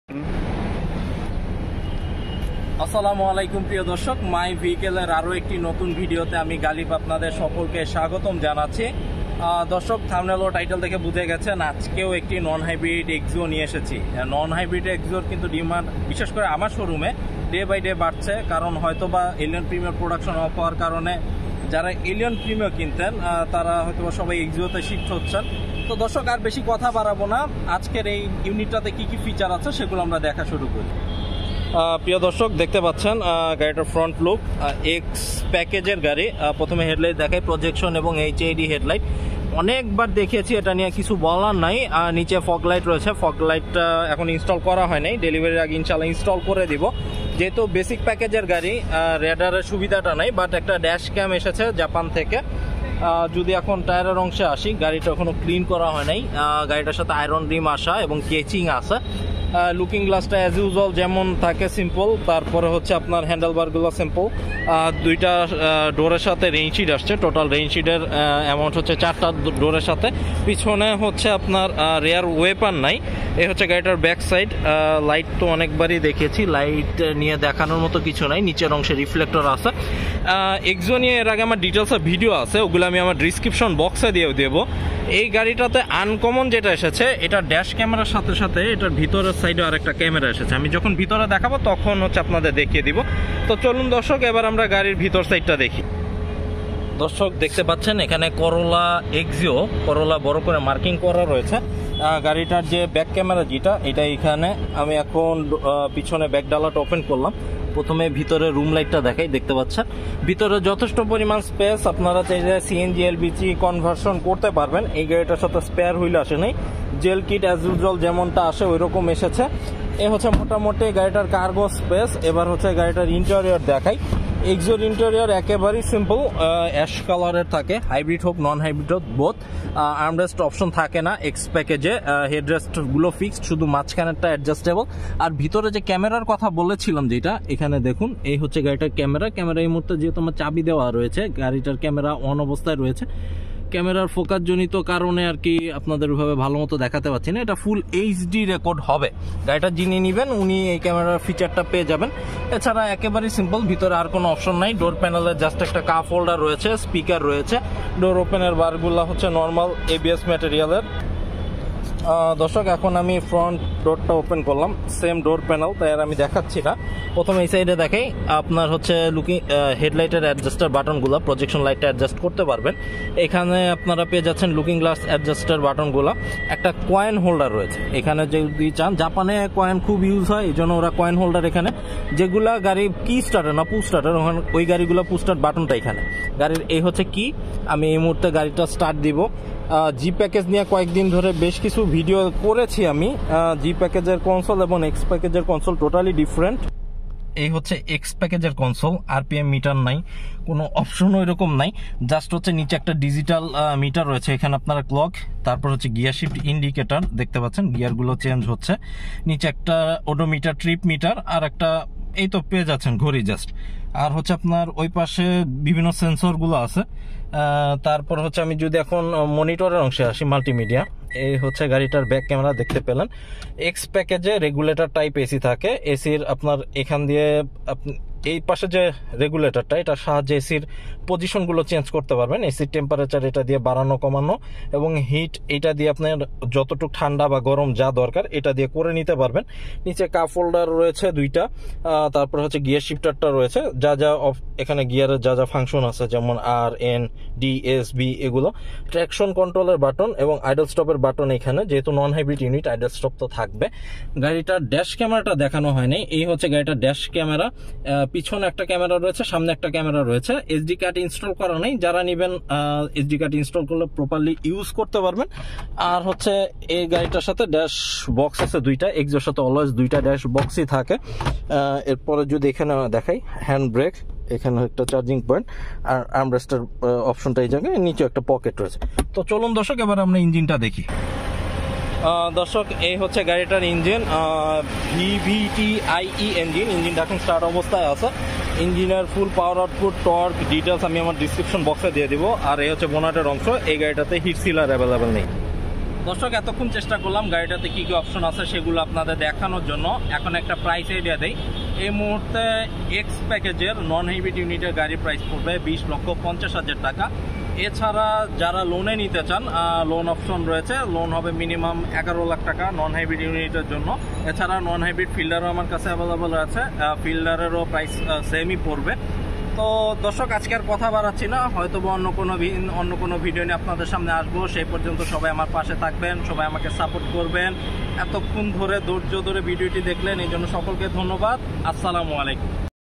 TOYOTA COROLLA AXIO NON-HYBRID X EDITION 2017 || বাংলা রিভিউ || OVERVIEW & PRICE FOR BANGLADESH
Presented by - NBN Automobiles & Kazi Global Trade
SHOWROOM ADDRESS :
📌 96, KA KURIL KAZIBARI DHAKA

Toyota Corolla Axio X Non-hybrid বাংলাদেশের অন্যতম জনপ্রিয় সেডান
এই ভিডিওটি মূলত বাংলাদেশীদের জন্য তৈরি করা হয়েছে

Toyota Corolla Axio X Edition
Mode                       : 2017
Engine Cap             : 1496 CC 16v 4-cylinder DOHC with electric motor
Transmission         : Electronic constant-velocity (e-CVT)
Horsepower            : 109 Bhp / 4800 rpm
Torque                     : 136 Nm / 4400 rpm
Body type                : 5-door sedan
Fuel                          : Gasoline (Petrol)
Fuel Tank                 : 36 L
Kerb Weight            : 1130 kg
Max. weight            : 1415 kg
Max load                  : 275 kg
Length                      : 4360 mm
Height                      : 1460 mm
Width                       : 1430 mm
Wheelbase              : 2600 mm
Country of origin    : Japan

All types of Japanese Car importer , Wholesaler & Retailer in Bangladesh
Video publish date: 11/10/2022